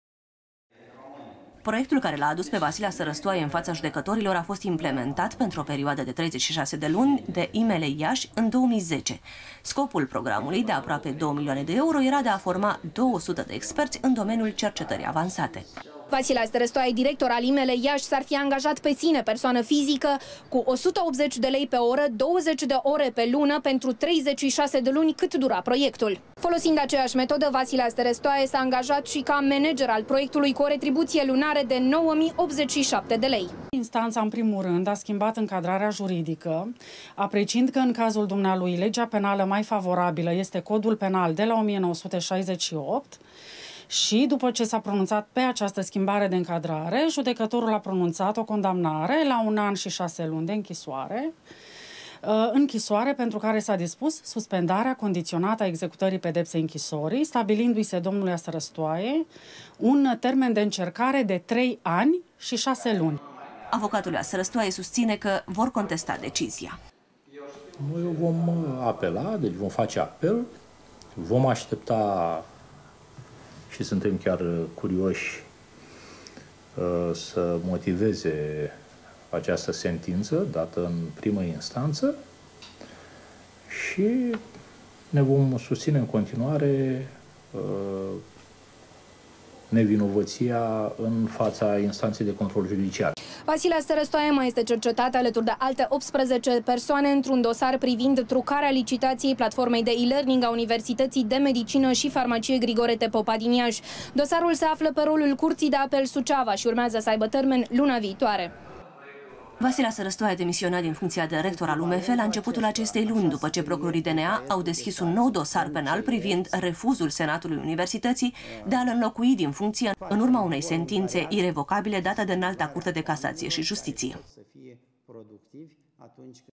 ASTARASTOAE CONDAMNAT DE PRIMA INSTANTA
Un an si 6 luni de inchisoare cu suspendare pentru fostul rector al UMF Iasi, Vasile Astarastoae. Este decizia in prima instanta in dosarul in care presedintele Colegiului Medicilor din Romania este acuzat de conflict de interese. Potrivit procurorilor, in 2010, Astarastoae s-ar fi angajat pe sine intr-un proiect european al Institutului de Medicina Legala Iasi, institutie la care atunci aera director.